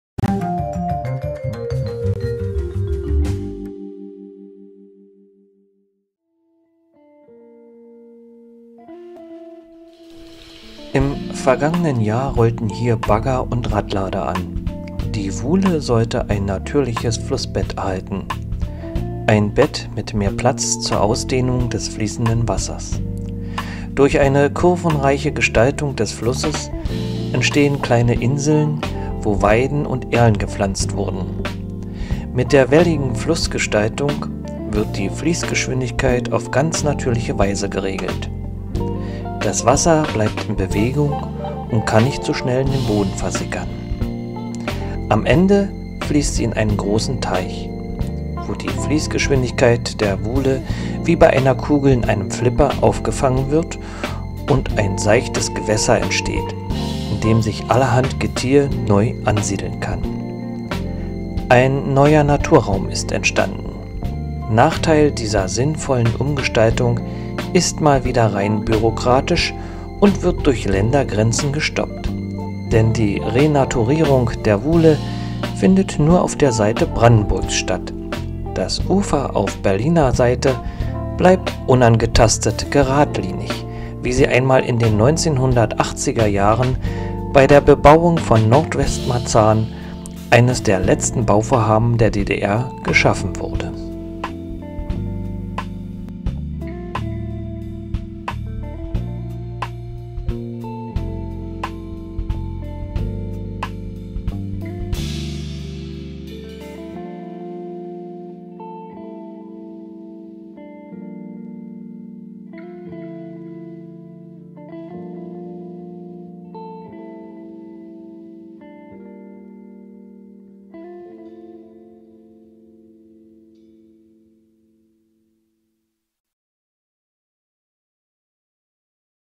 Die Renaturierung der Wuhle
Im vergangenen Jahr rollten hier Bagger und Radlader an.
Die Wuhle sollte ein natürliches Flussbett erhalten. Ein Bett mit mehr Platz zur Ausdehnung des fließenden Wassers. Durch eine kurvenreiche  Gestaltung des Flusses entstehen kleine Inseln, wo Weiden und Erlen gepflanzt wurden. Mit der welligen Flussbettgestaltung wird die Fließgeschwindigkeit auf ganz natürliche Weise geregelt. Das Wasser bleibt in Bewegung und kann nicht so schnell im Boden versickern. Am Ende fließt sie in einen großen Teich, wo die Fließgeschwindigkeit der Wuhle wie bei einer Kugel in einem Flipper aufgefangen wird und ein seichtes Gewässer entsteht, in dem sich allerhand Getier neu ansiedeln kann.
Ein neuer Naturraum ist entstanden.
Nachteil dieser sinnvollen Umgestaltung ist mal wieder rein bürokratisch und wird durch Ländergrenzen gestoppt, denn die Renaturierung der Wuhle findet nur auf der Seite Brandenburgs statt. Das Ufer auf Berliner Seite bleibt unangetastet geradlinig, wie sie einmal in den 1980iger Jahren bei der Bebauung von Nordwestmarzahn, eines der letzten Bauvorhaben der DDR geschaffen wurde.

Ein Bericht von Alexander Papritz